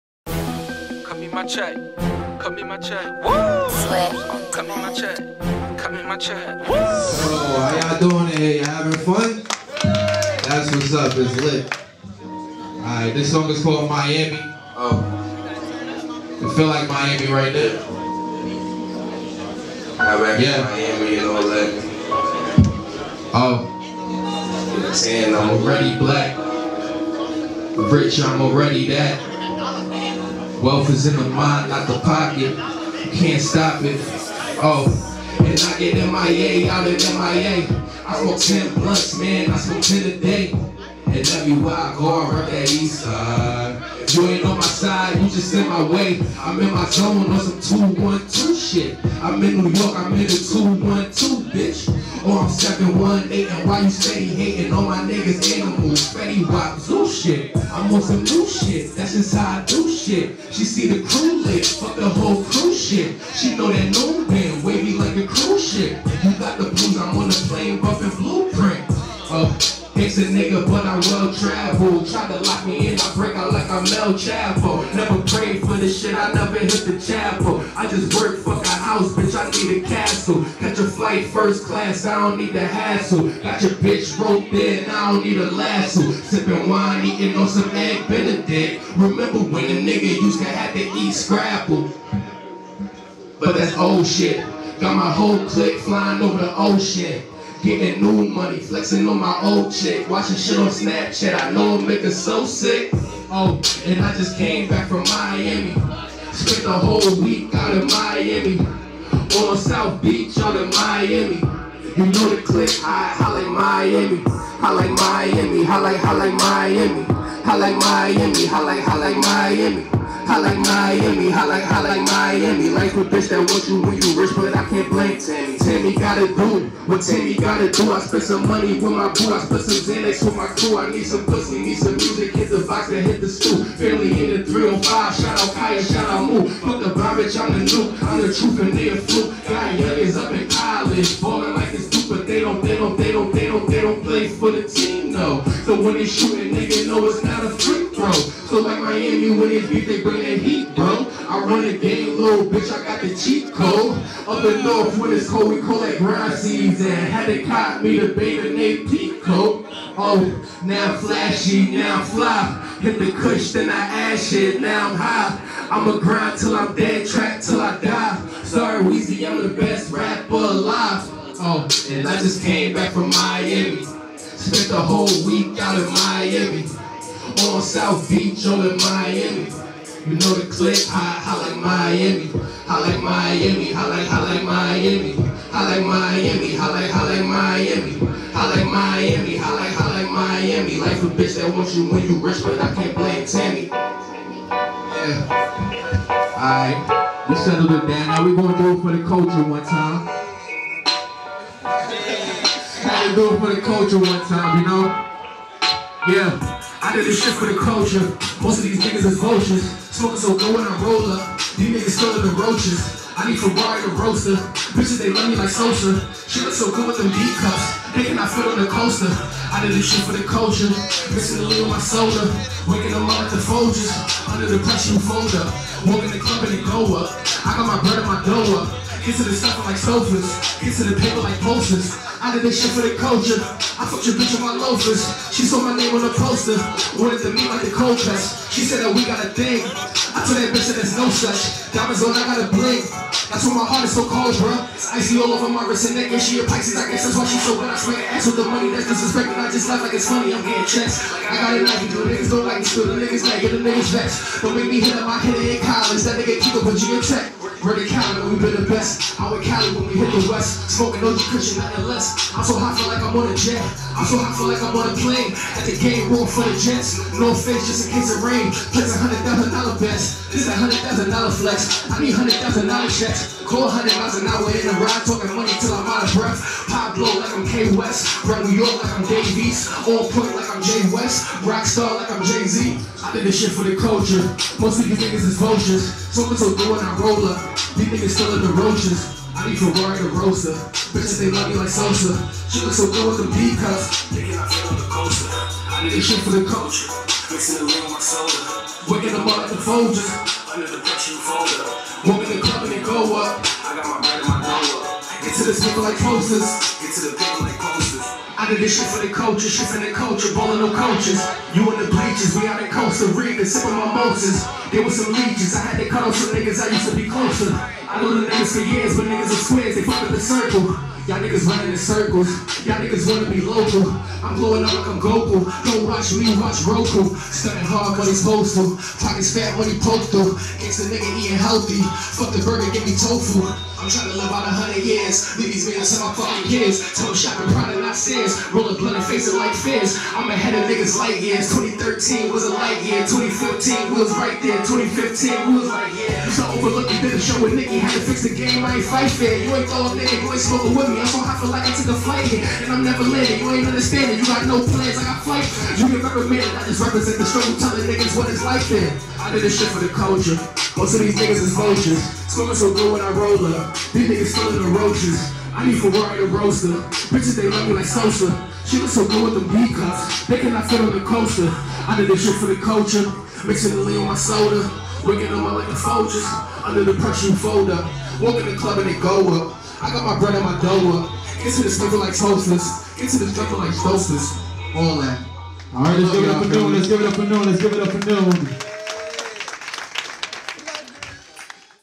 GRINDHOUSE SHOWCASE: JSIADI NOON
Grind House Showcase @LyricsLounge
here going for the prize 10hr Block recording and internet radio play and a chance to open for SwiftOnDemand is Jsiadi Noon
video by David Immortal